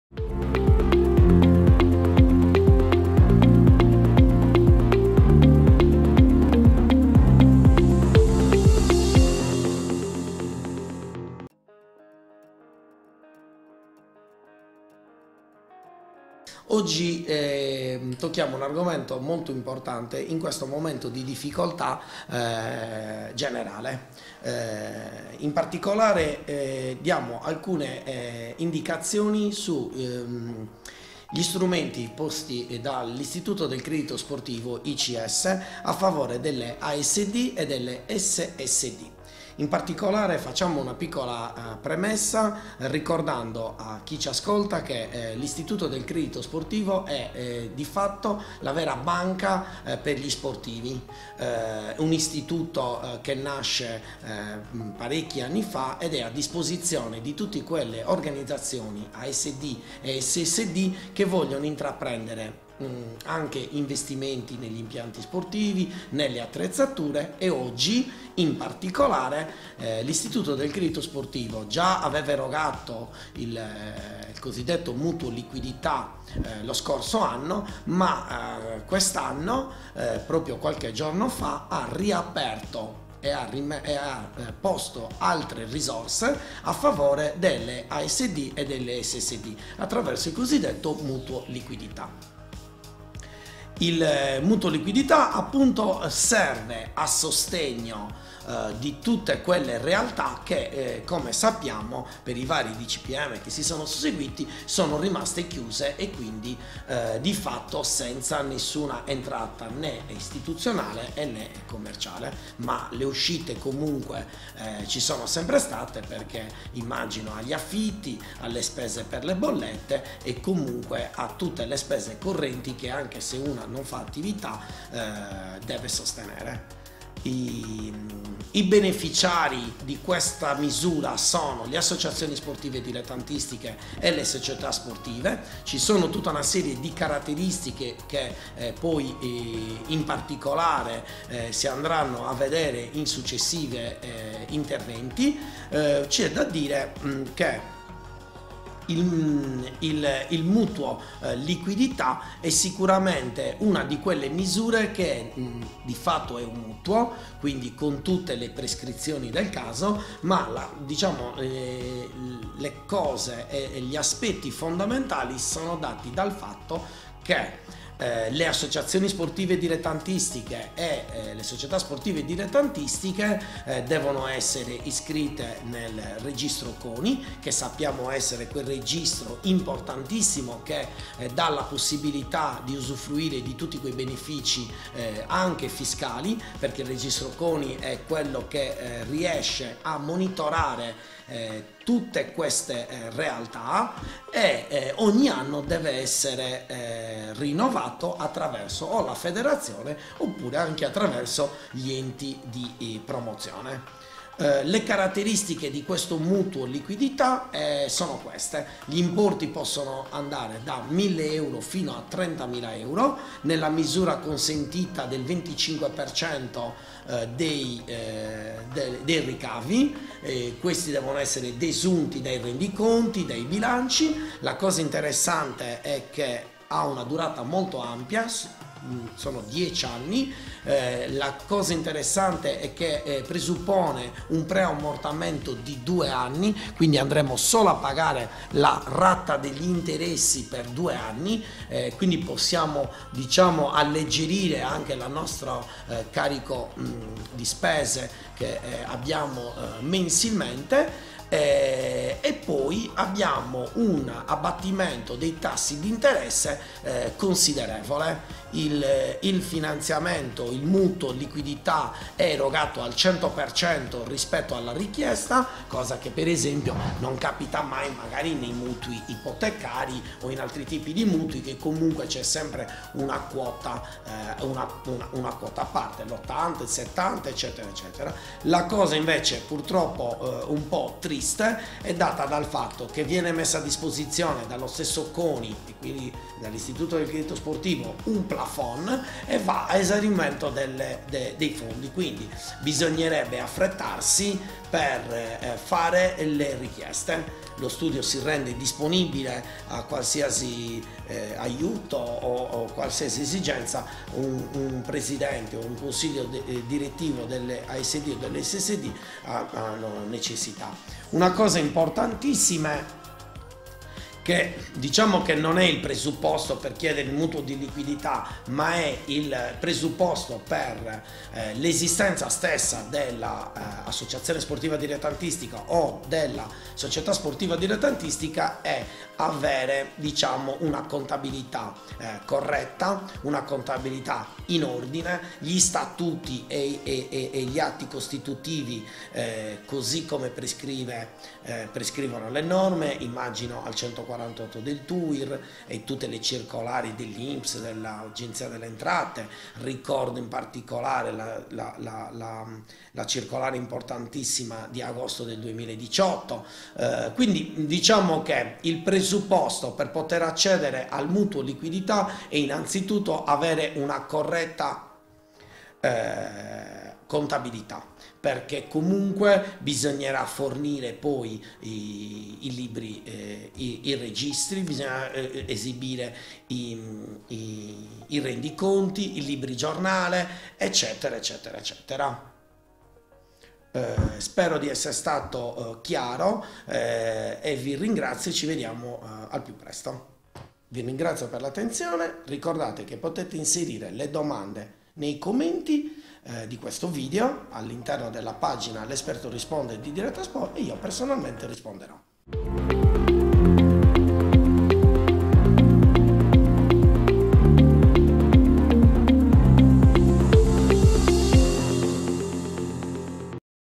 L'Esperto Risponde - Mutuo Liquidità per ASD e SSD - Cosa è, come richiederlo
Uno strumento fondamentale è a disposizione delle ASD e SSD in questo momento di gravi difficoltà economiche.

E' il Mutuo Liquidità! 💶💶💰💰

Scopri cosa e come funziona nella prima puntata del 2021 della rubrica LEspertoRisponde, curata dal Dott. Massimiliano Testa,commercialista esperto nel fiscalismo e nella gestione delle associazioni e società sportive dilettantistiche.

Giovedì 11 febbraio, ore 17, Studio Testa, via Rockfeller, Cagliari
immagini di Mauro Farris, a cura del Dott. Massimiliano Testa